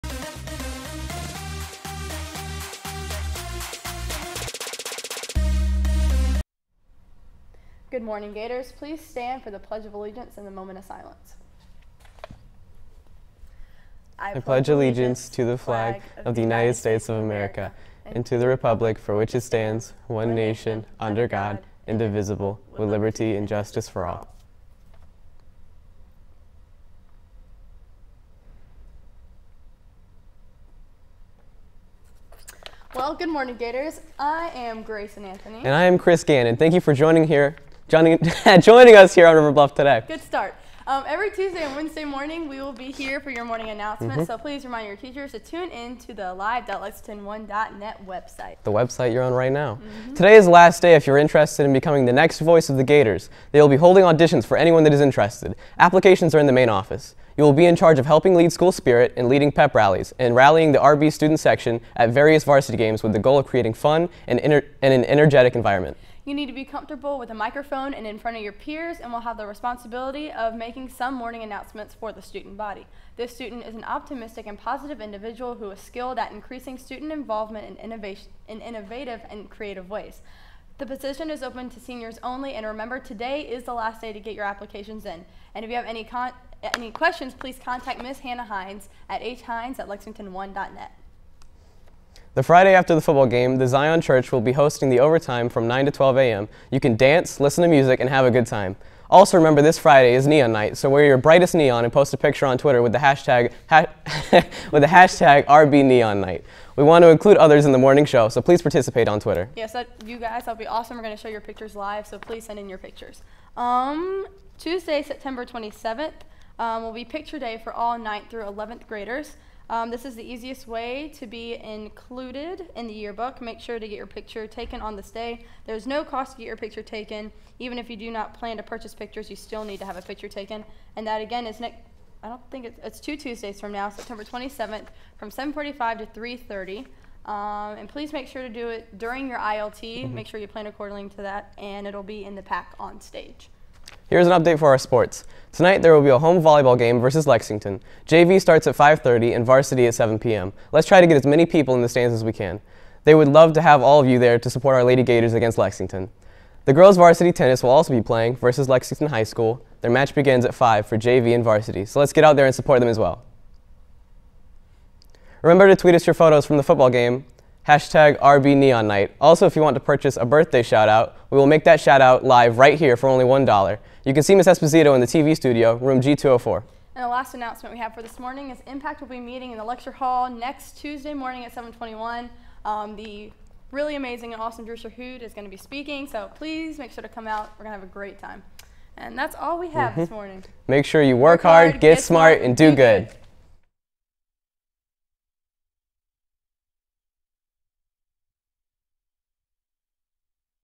September 13, 2016; River Bluff Today Morning Announcements
River Bluff High School, Center for Media Arts Design and Production Media Technology Class. September 13, 2016 Live Announcements.